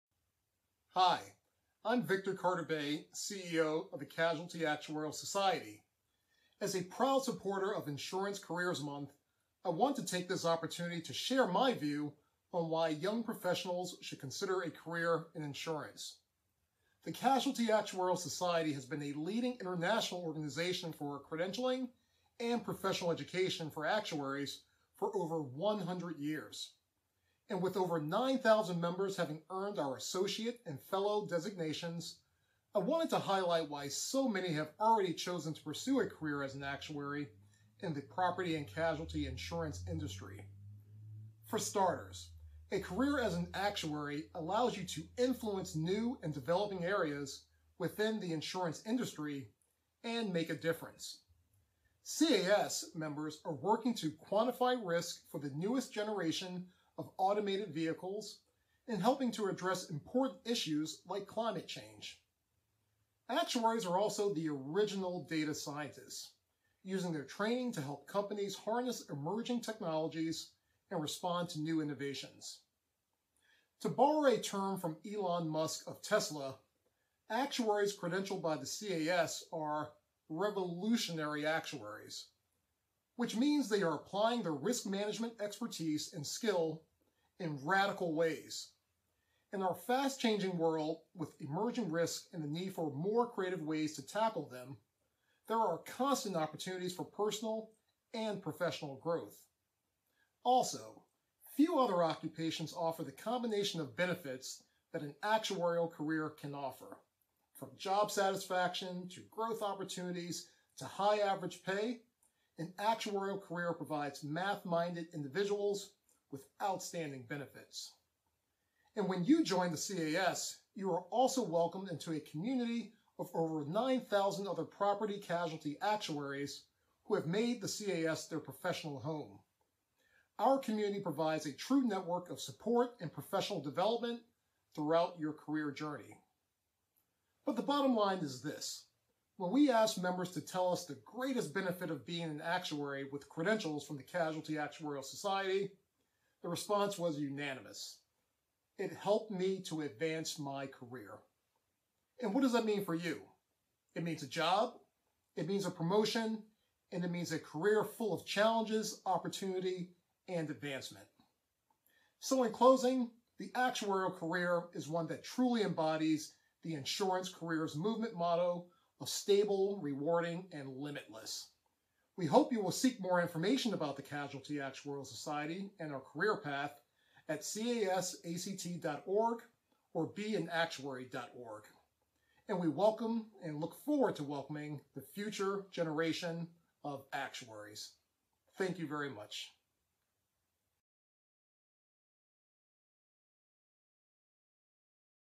CAS CEO on Insurance Careers Month
Victor Carter-Bey, CEO of the CAS, shares why young professionals should consider a career in actuarial science.